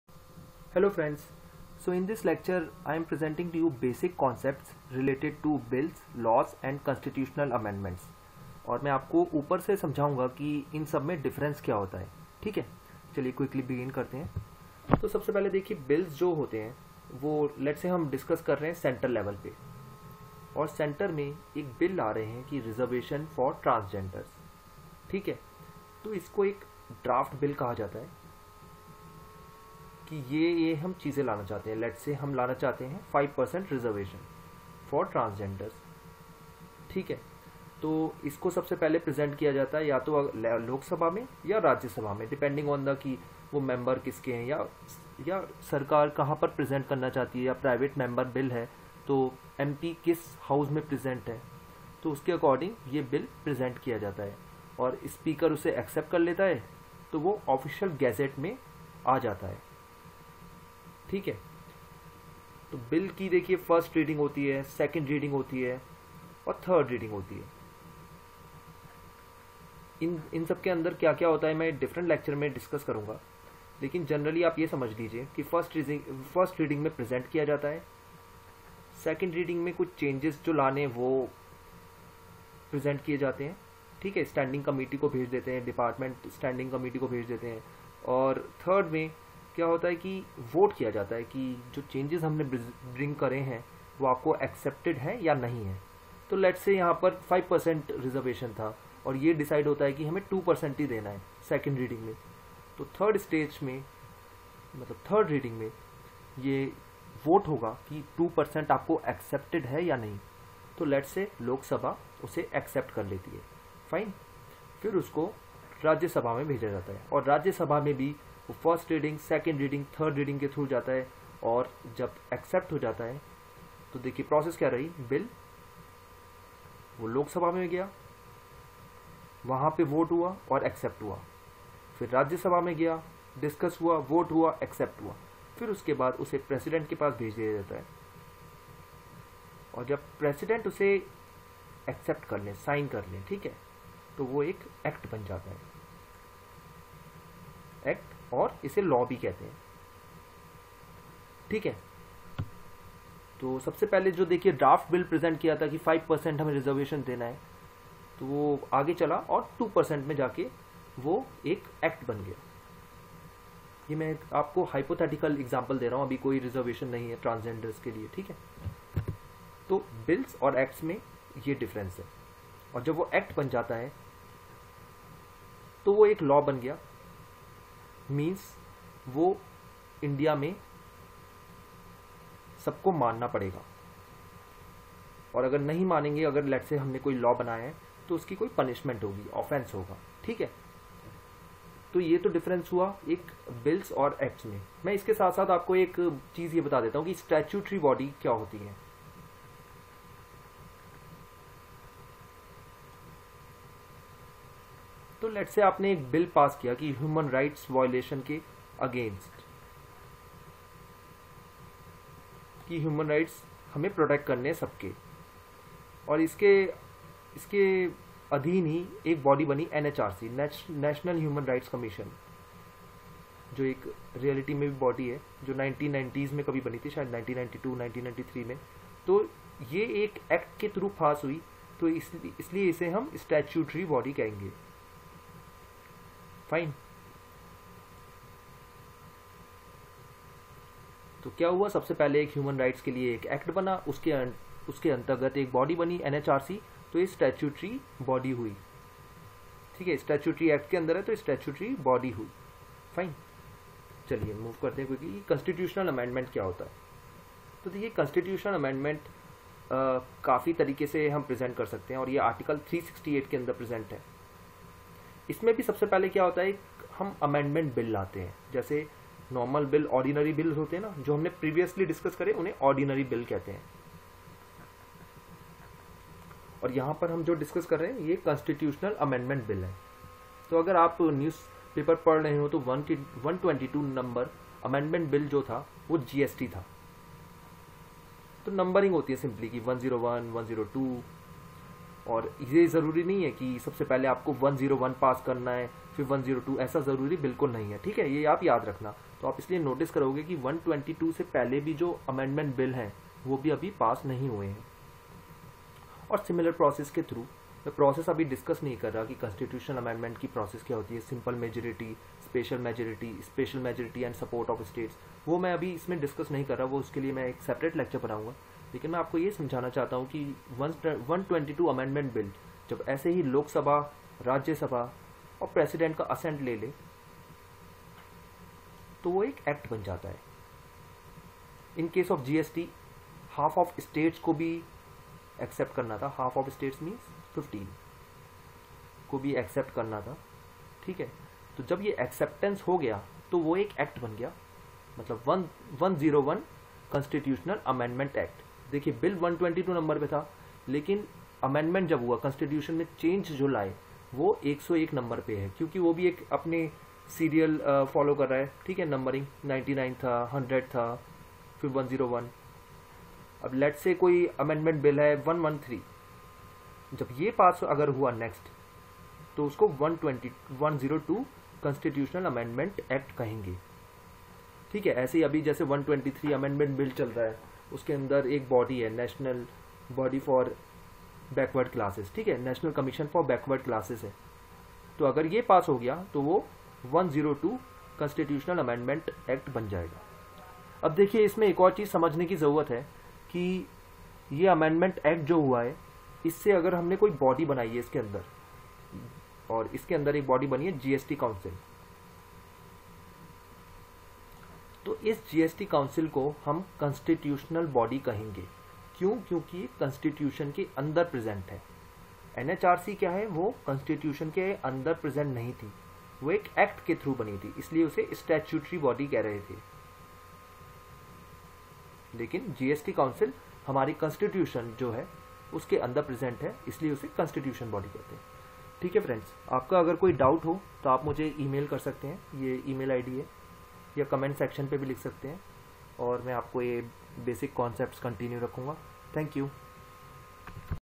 Bills Acts Constitutional Amendments Basics
This video contains basics of bills , acts , constitution amendment acts, statutory body and constitutional body.